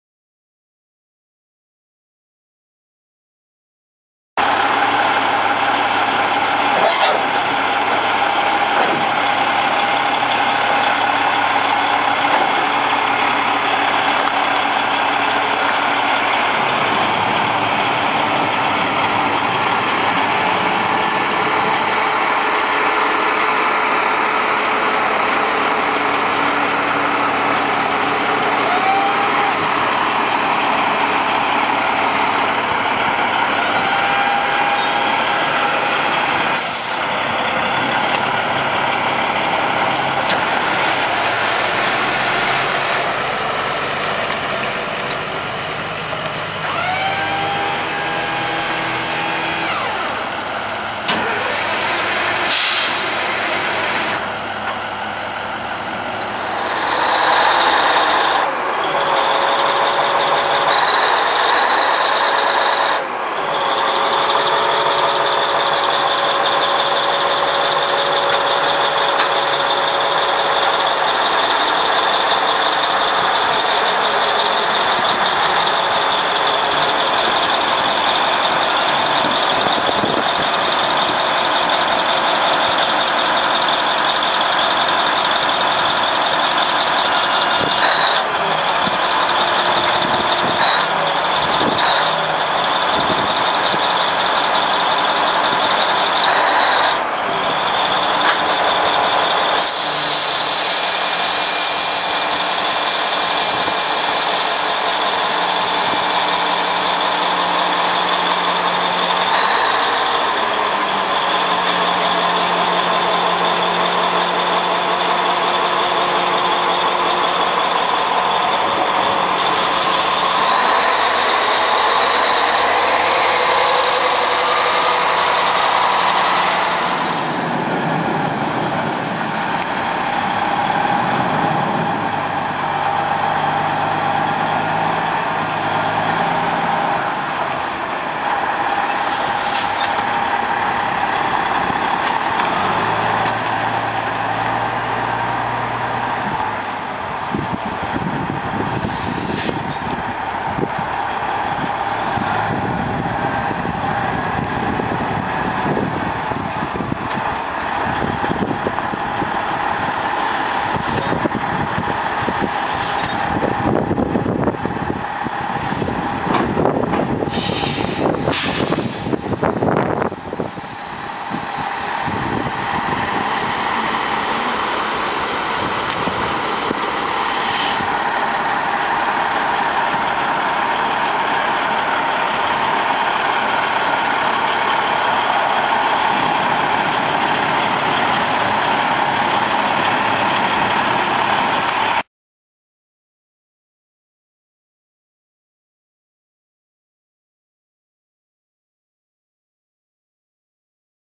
kinney towing
My buddy Roland who drives for Kinney Towing showed up with his awesome Petebilt. He was moving our old Cat 769 from the quarry to the plant.